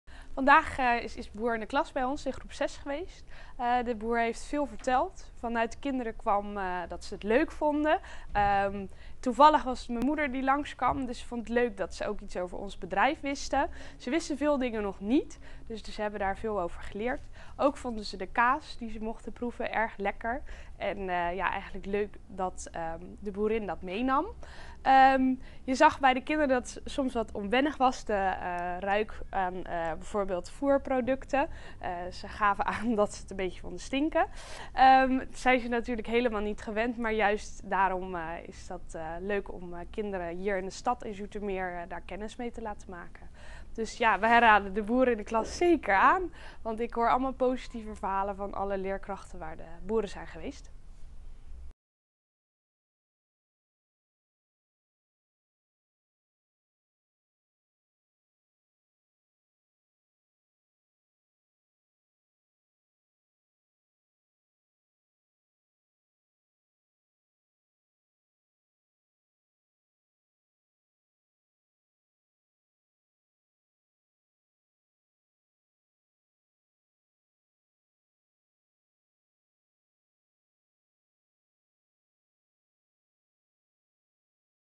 Review Femke Ammerlaan Zoetermeer
Femke Ammerlaan vertelt hoe 8 groepen op basisschool De Oostwijzer in Zoetermeer het ervaren hebben om Boer in de Klas te ontvangen en zo praktijkles kregen wat een melkveehouder doet, hoe een kalf opgroeit tot koe en welke eten op je bord van een boerderij komt.

Tot en met 19 april kunnen scholen een subsidie aanvragen 'Lekker naar Buiten' om deze gastlessen te financieren.

Een interview met basisschoolleerkracht & gastboerin Maud en Ankie
van Houts over Boer in de Klas is te vinden op:

Je kan dit bericht op Facebook en LinkedIn delen

Team Boer in de Klas Ankie van Houts en Jacoline Peek-Hamoen